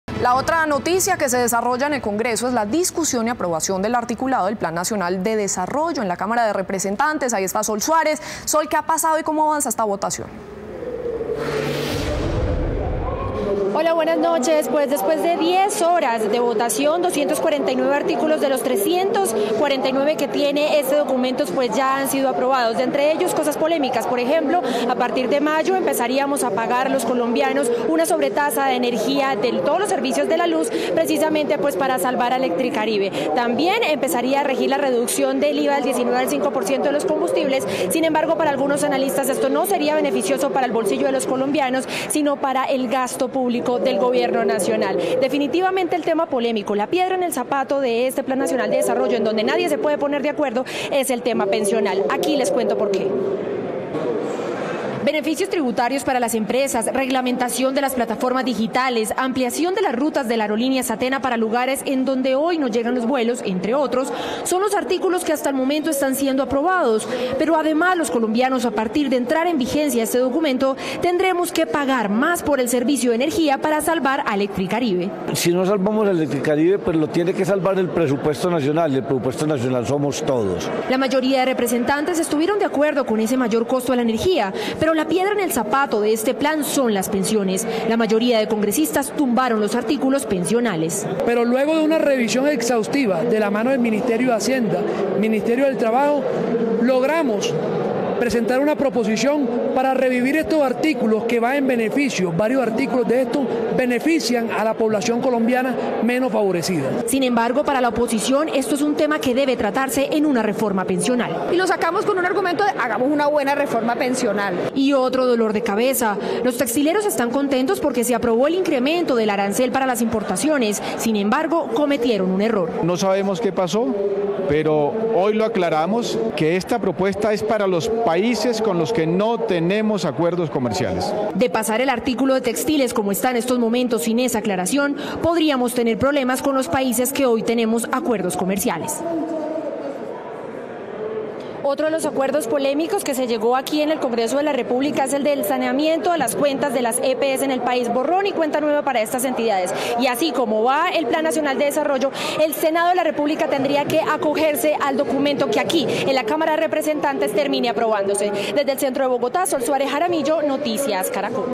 Pensiones siguen siendo la piedra en el zapato en votaciones del PND | Noticias Caracol
Tras 10 horas de votación, 249 de los 349 artículos ya han sido aprobados. Algunos polémicos, como la sobretasa a la energía.